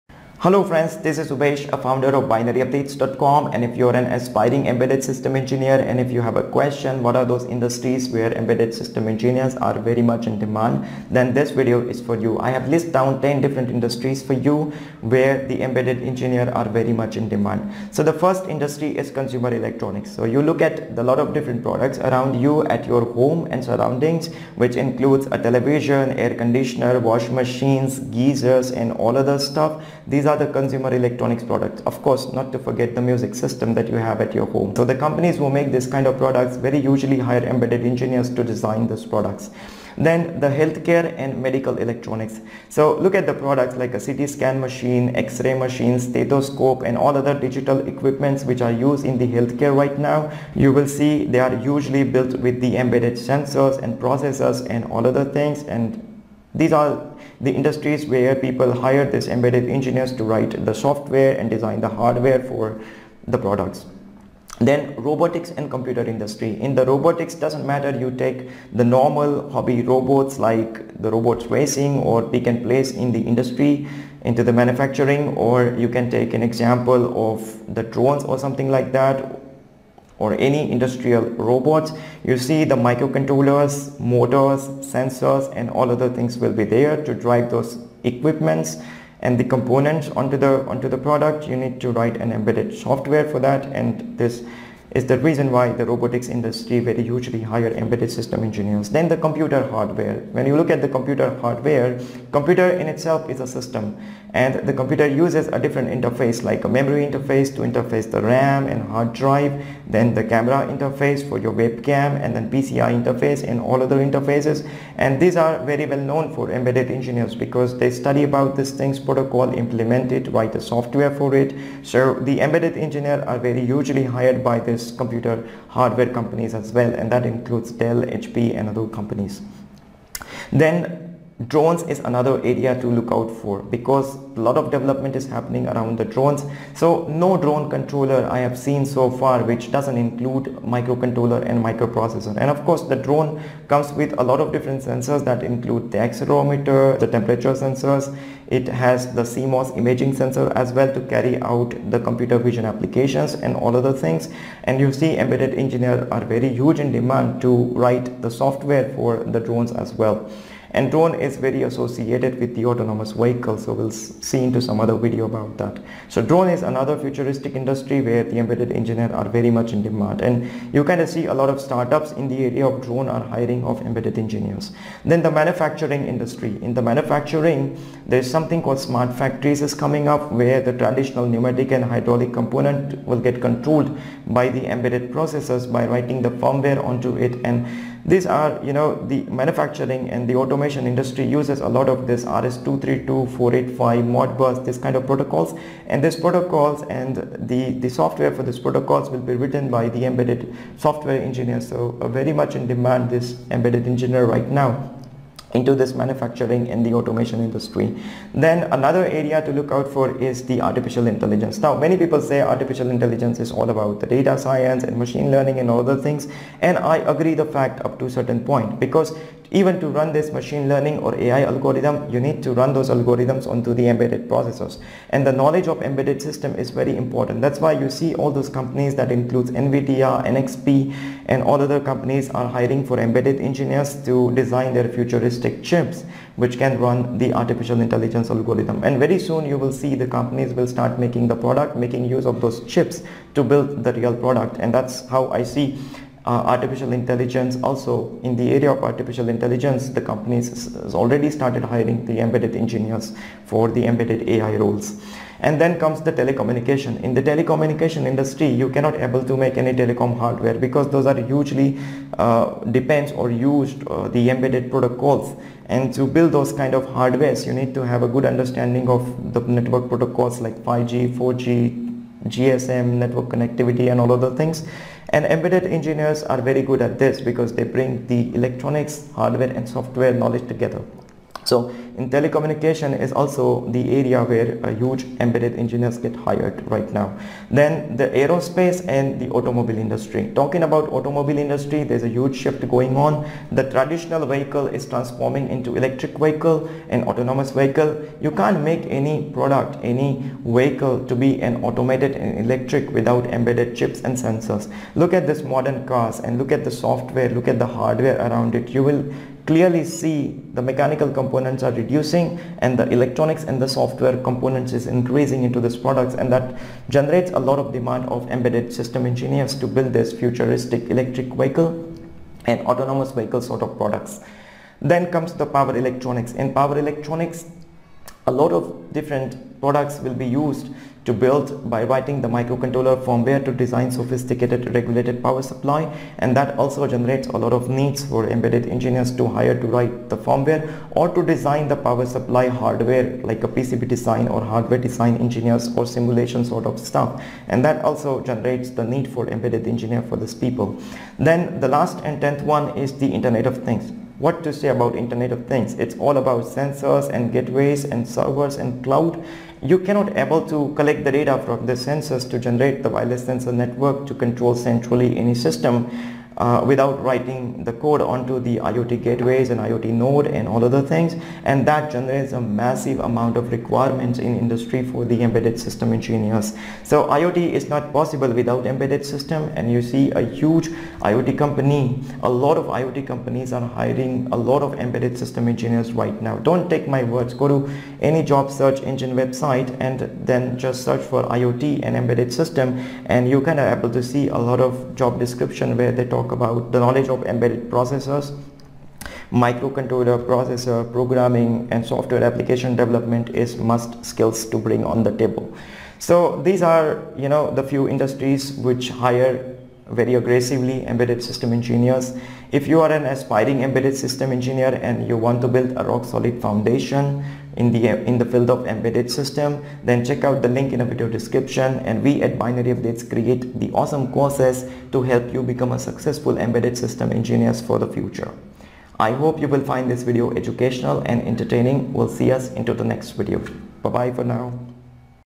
Top-10 Industries for Embedded Systems Engineer- Job Opportunities
This video will walk you through Top-10 Industries for Embedded System Engineers to find Job Opportunities. If you are aspiring Embedded Engineer. Here is an list of industries or companies to explore for better career and job opportunities
1 Consumer Electronics
2 Healthcare or Medtronics
3 Robotics and Computer industry
4 Dones & Robotics
5 Manufacturing & Automation industry
6 Artificial Intelligence- Embedded AI
7 Telecommunication
8 Aerospace and automobile
9 Power Electronics
10 Internet of Things and list goes on and on...

You'll find them in almost all sectors and industries since every other product these days uses microchips and sensors. The roles and responsibilities also largely depends on whether company whether its service base or product base.

►►Learn to Solve Real Problem and Become Expert Embedded Systems Professional

Umesh Lokhande
BINARYUPDATES.COM